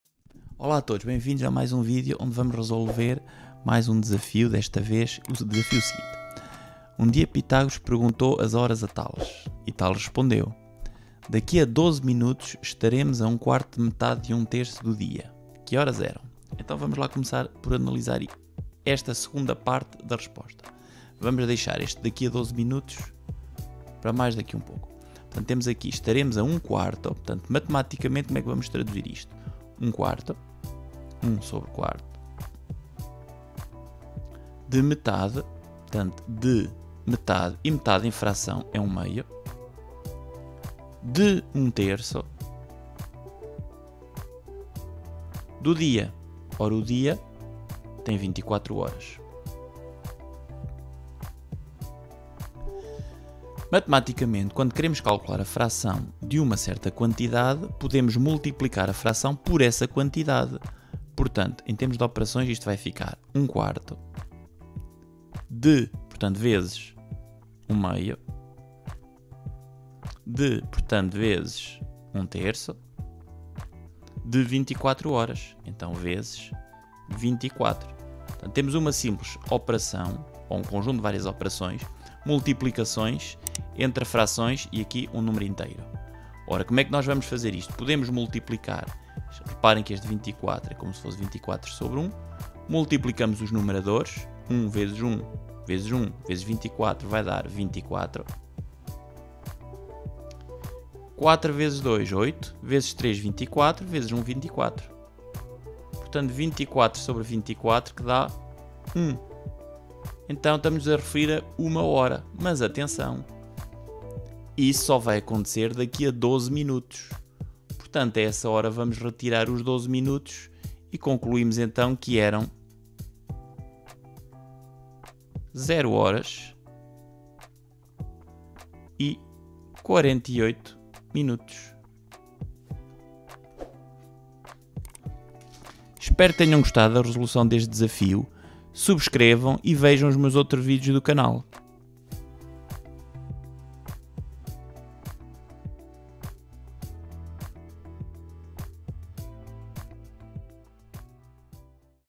CONSEGUES RESOLVER O DESAFIO DE TALES A PITÁGORAS???
Desafio super simples que envolve frações, horas e minutos!